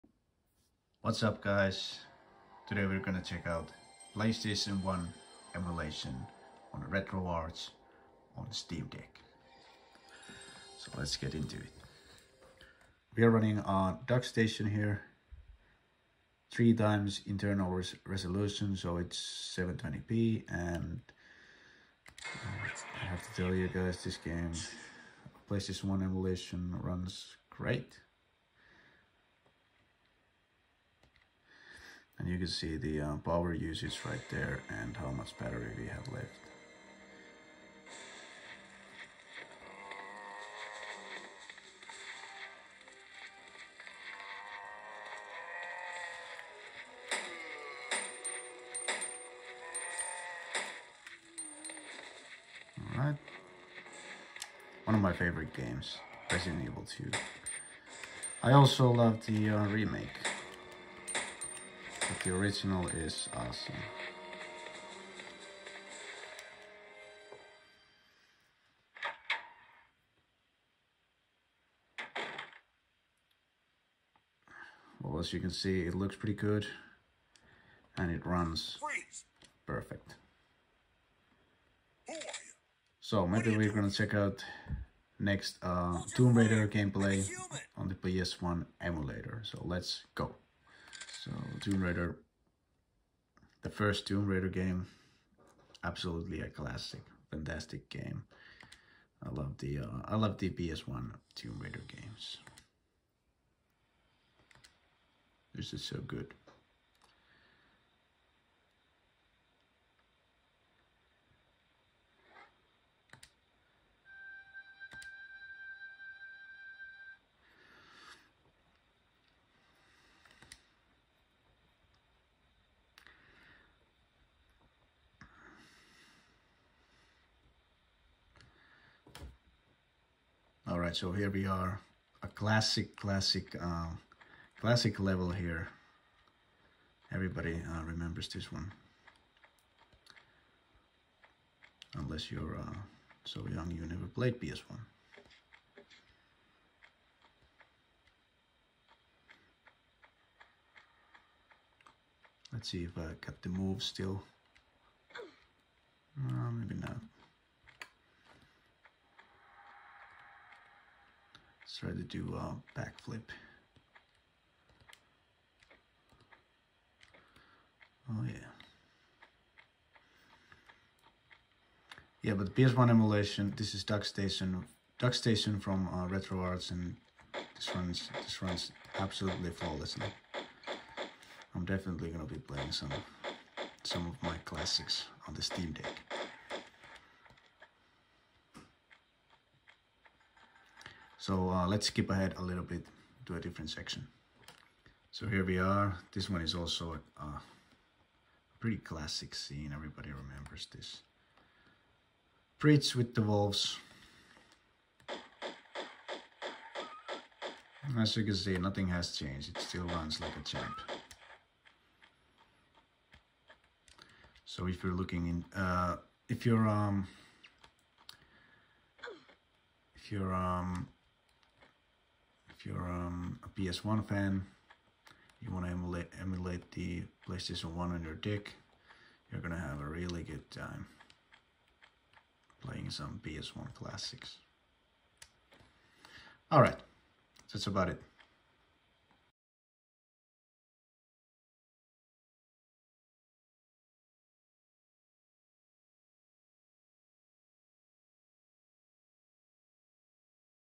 Steam Deck Retroarch PS1 Emulation
Quick video showing how retroarch ps1 emulation works with steam deck . Performance is great and battery usage very low so fantastic results